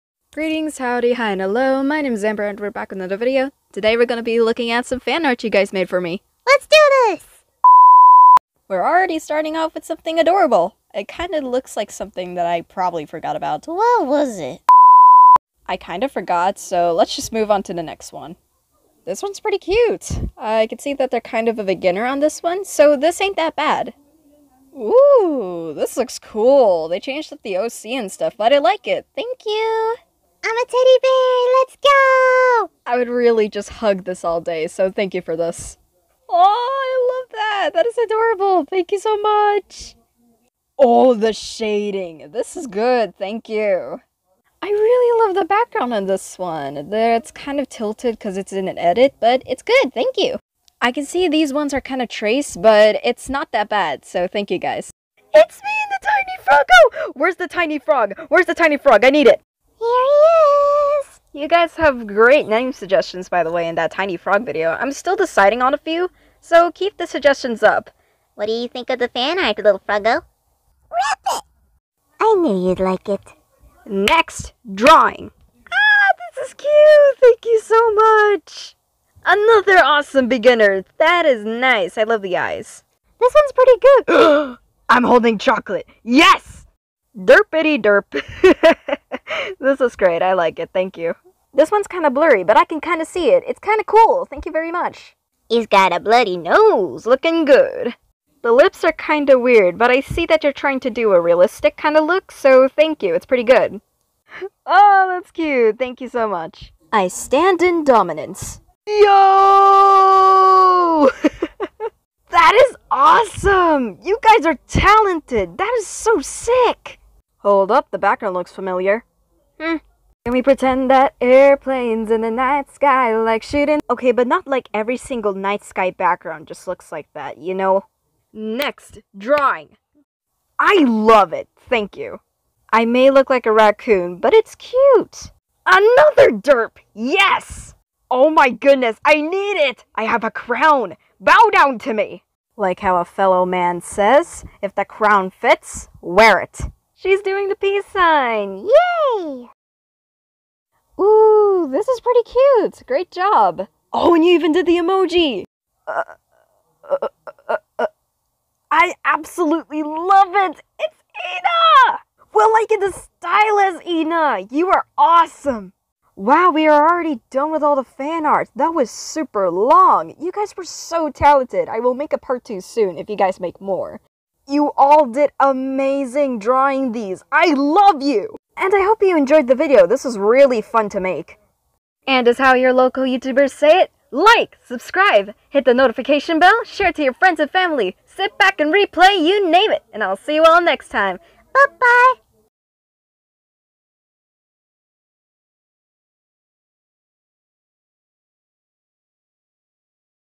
Reacting to some FanArt you all made! :D
I hope you guys have enjoyed this! I made a playlist with everyone who made the fanart! (Shout out to them, btw!)